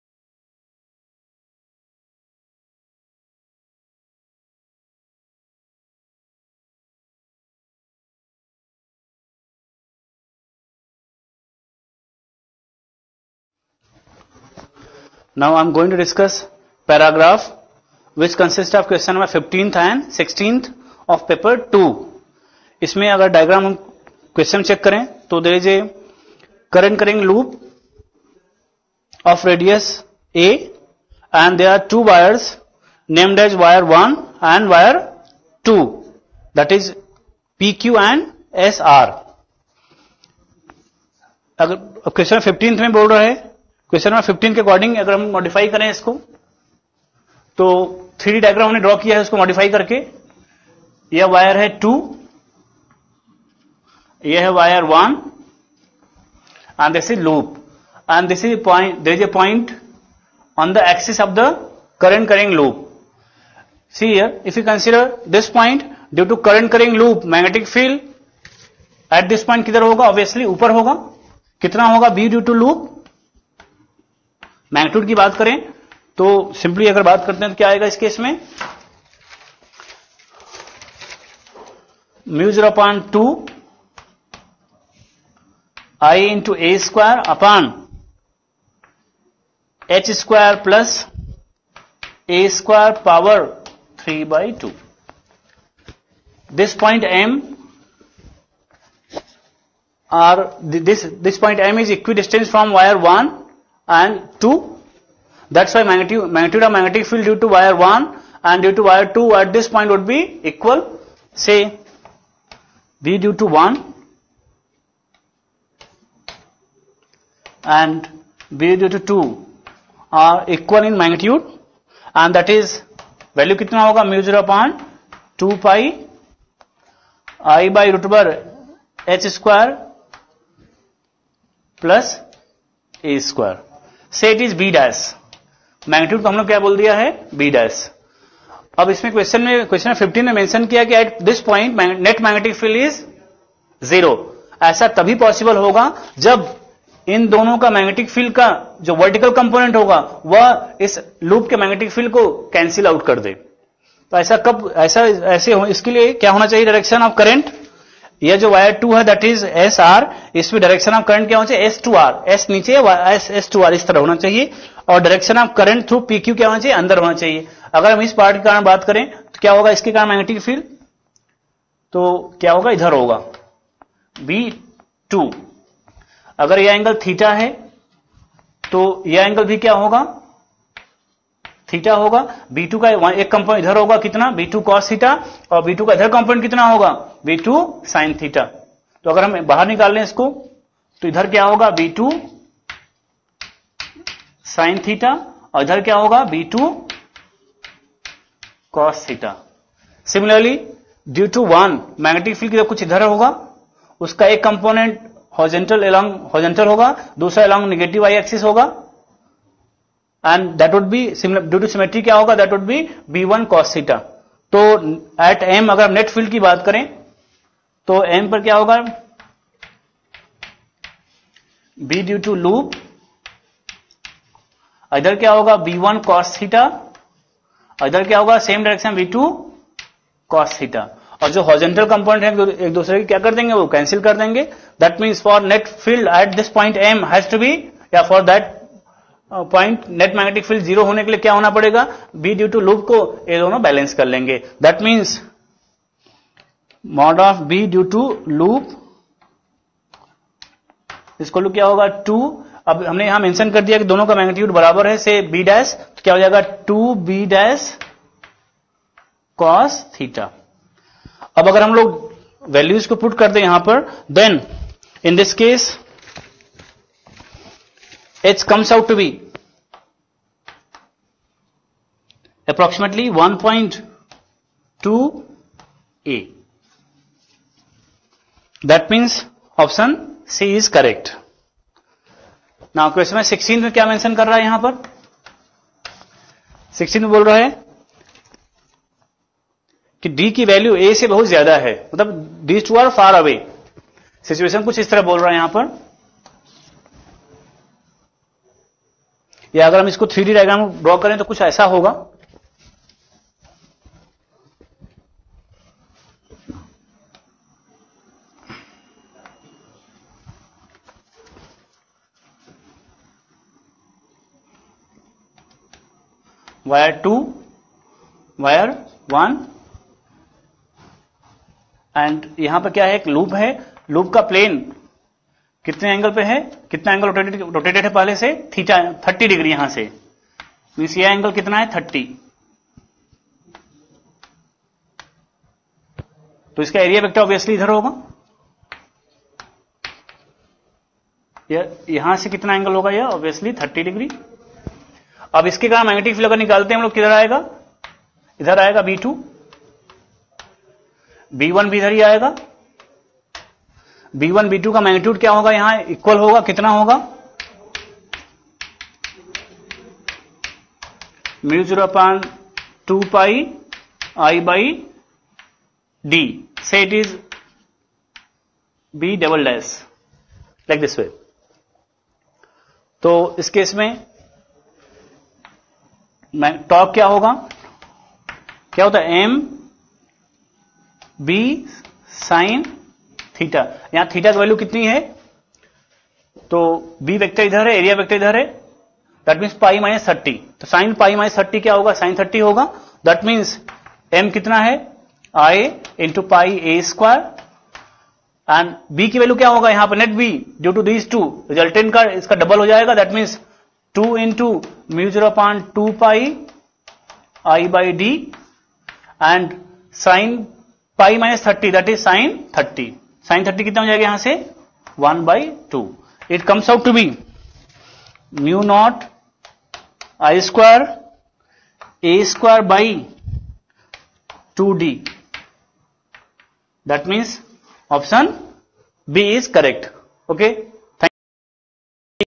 JEE-Advanced 2014 Physics- Q. 15&16 [Paper-II] [Code-5]
JEE-Advanced 2014 Video Solutions of Physics Paper by Career Point Expert Faculty.